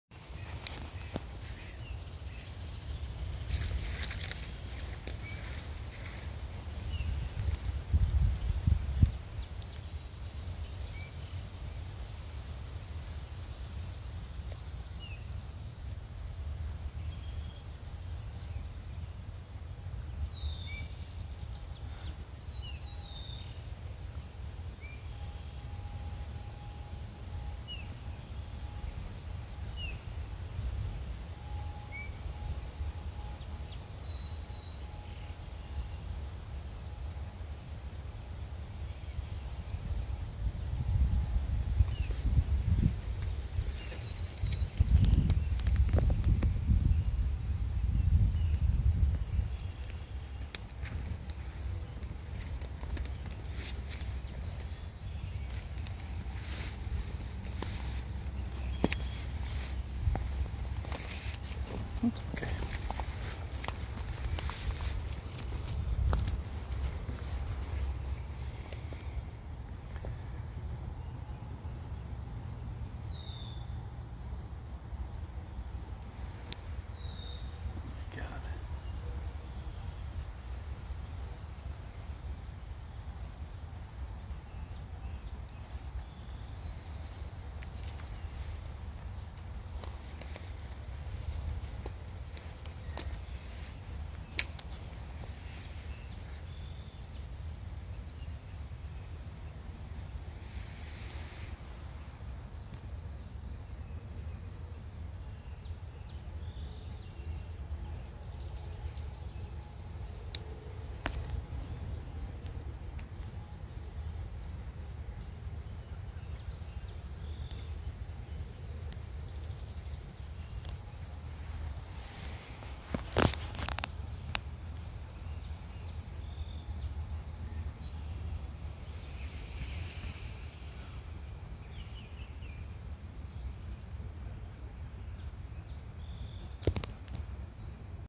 What A Surprise! Walk in the Woods
What looks like a gut pile in this video is actually the placenta from the birth that happened just moments before! After I left, the mother came to nurse the babies and eat the placenta. I also didn't know there was more than one fawn at the beginning of the video, so when I move the camera to spot the other two, that is the moment of discovering they are twins - no - triplets!
I didn't notice these whitetail deer fawns until I was already among them. They have such great camouflage when they hold still! There is nothing wrong with them. They were born just minutes before the video was taken, and this one came to me with its first steps. These are wild animals in their natural habitat, so I did not intervene.